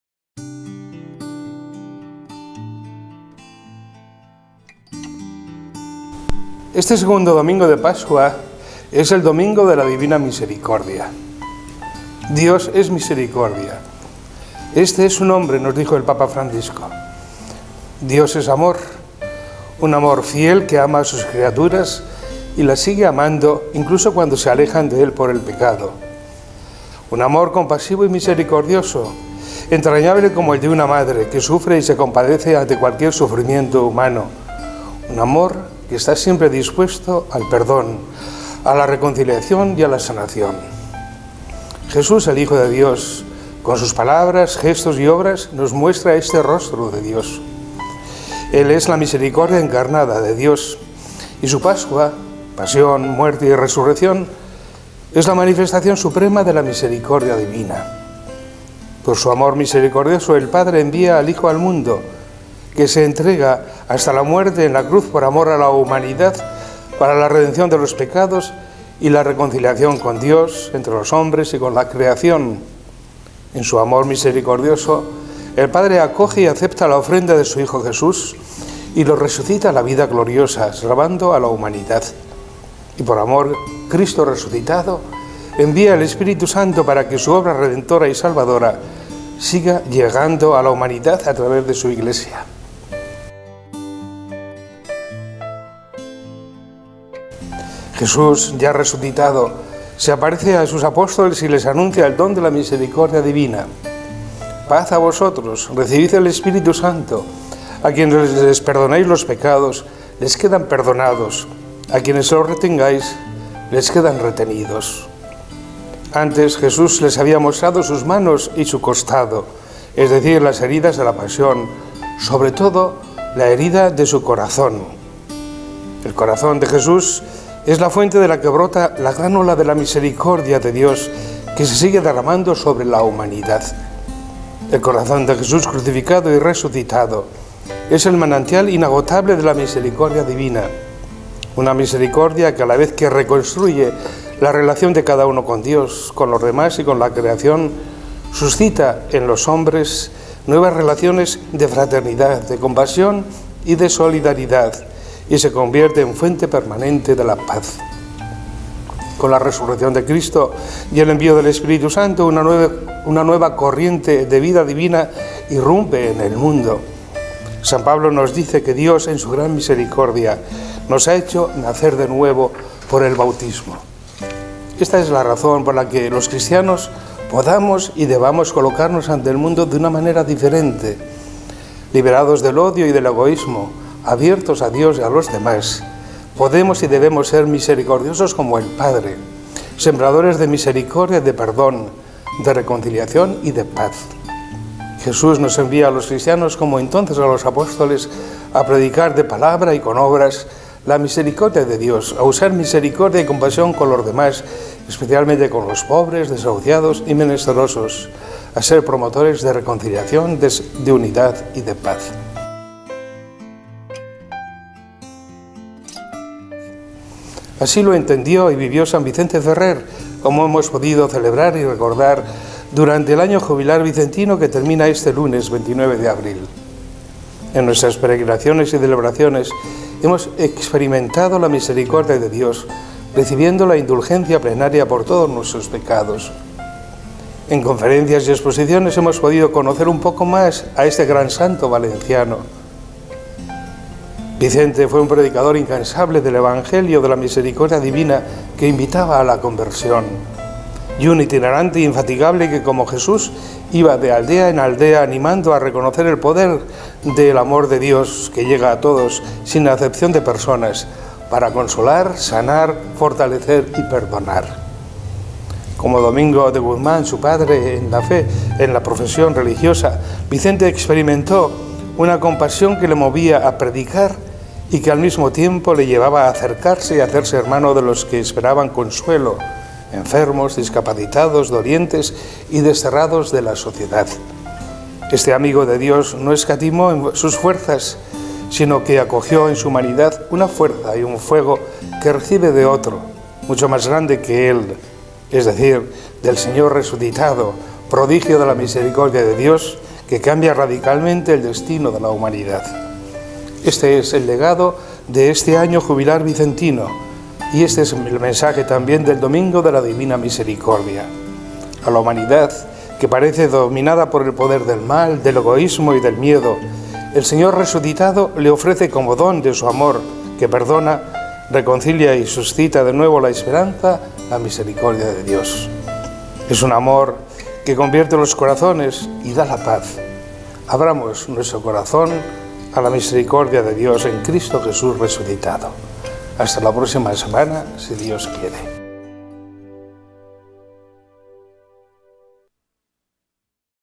San Vicente, predicador y testigo de la Misericordia
Carta de Mons. Casimiro López Llorente en el Domingo de la Misericordia, el 28 de abril de 2019, y al final del Jubileo por el VI Centenario de la muerte de San Vicente Ferrer.